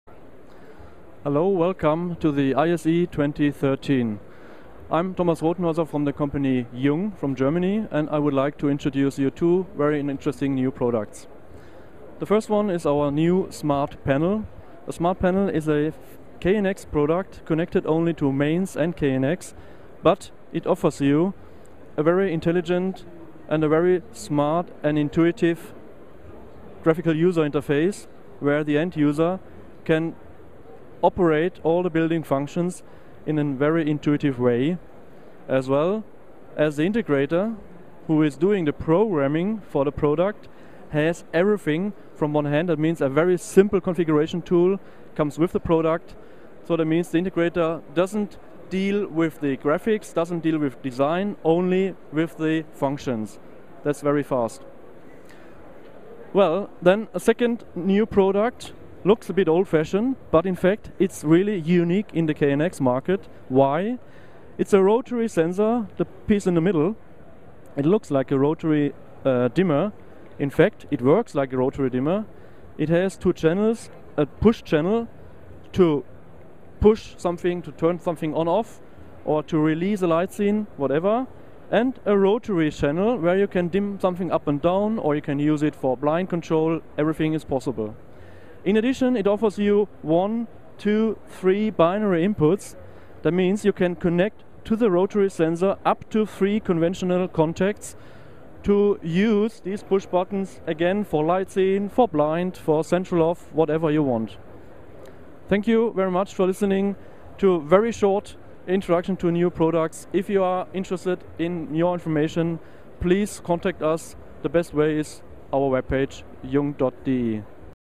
ISE 2013: Jung Displays its KNX Smart Panel and Rotary Sensor
Jung Displays its KNX Smart Panel and Rotary Sensor